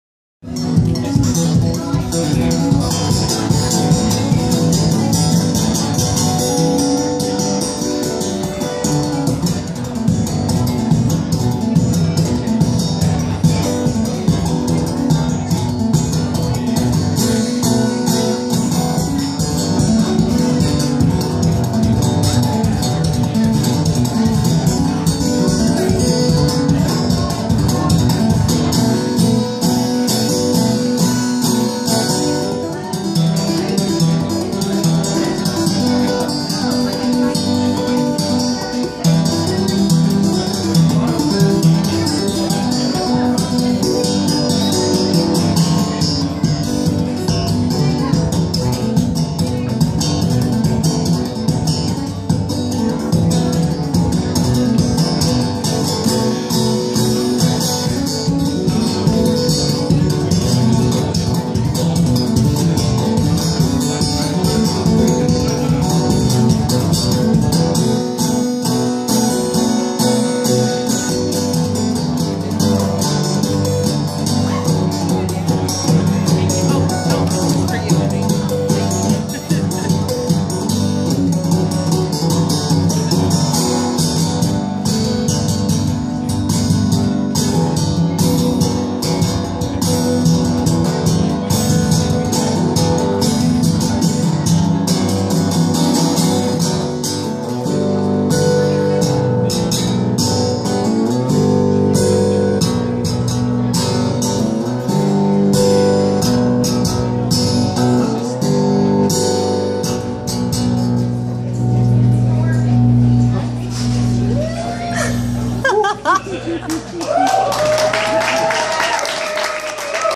Maxwell Hughes jammin' at Piper Down
Maxwell Hughes of the Lumineers jammin' on acoustic guitar